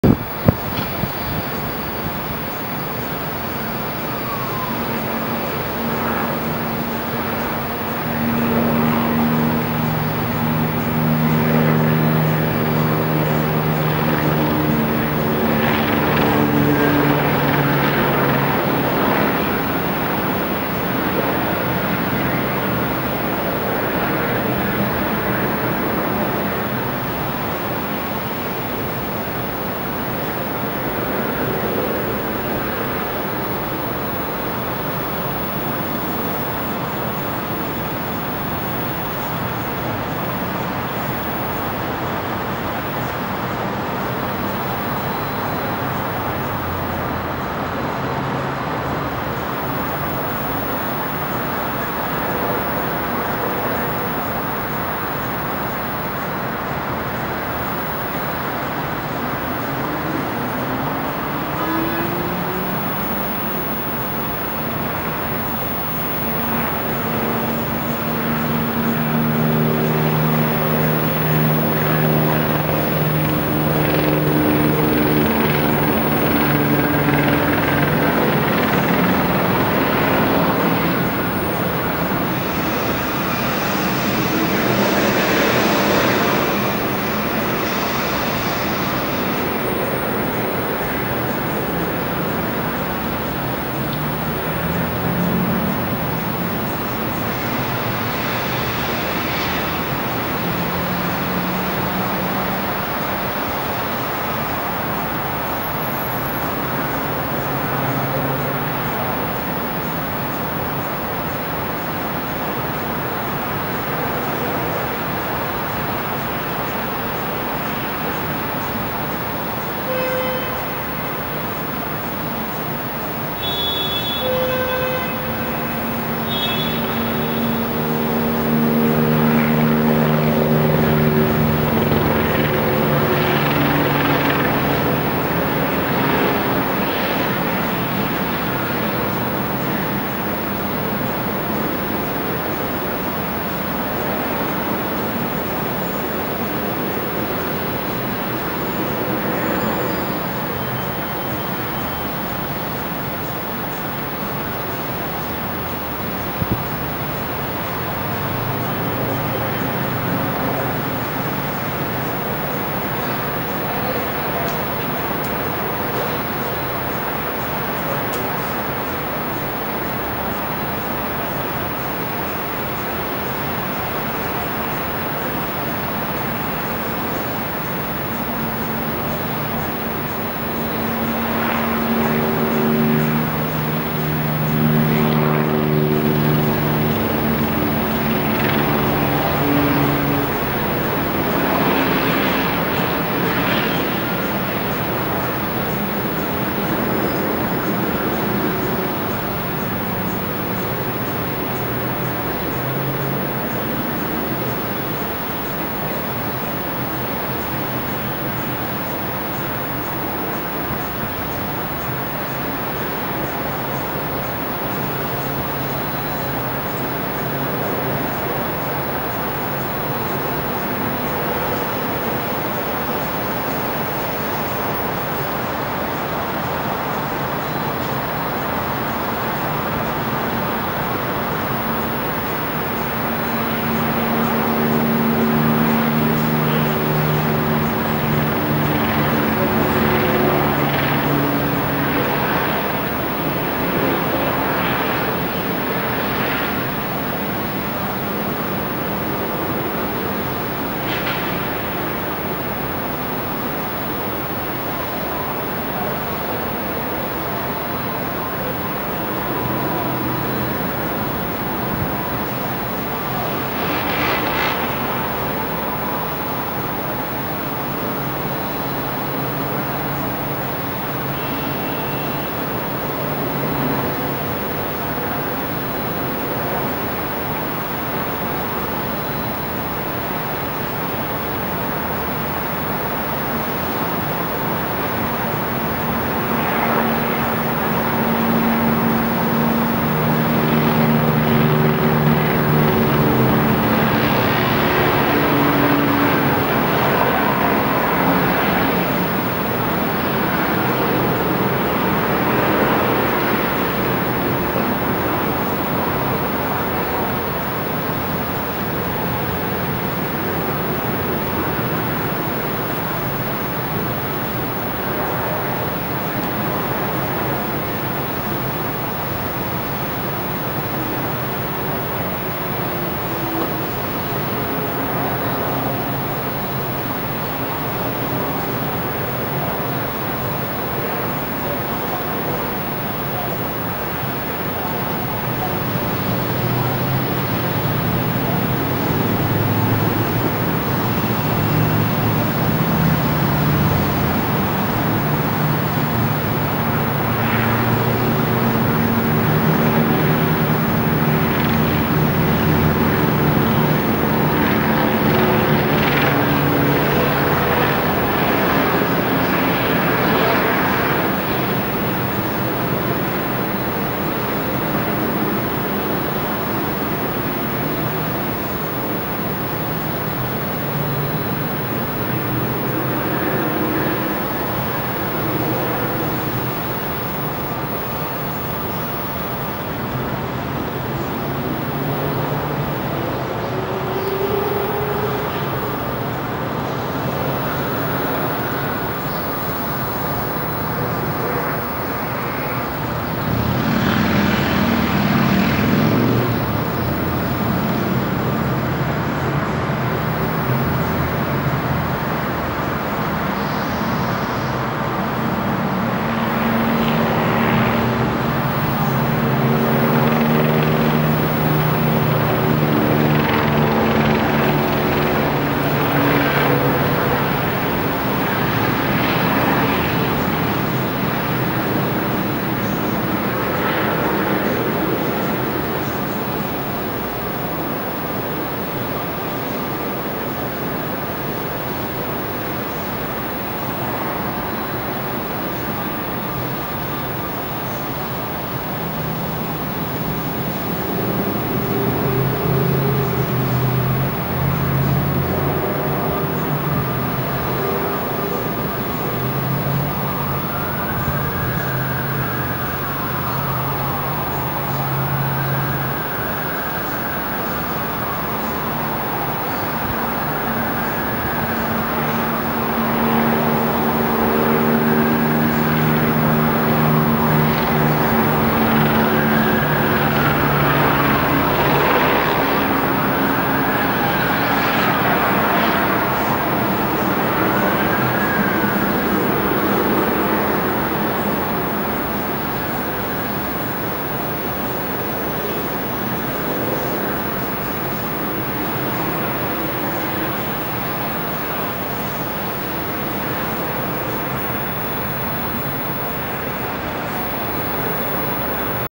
Structure Fire Rainey Street District Austin
Large structure fire in an abandoned structure in the Rainey Street District in Downtown Austin on March 11, 2015. The fire was in a building in the 500 block of Willow Street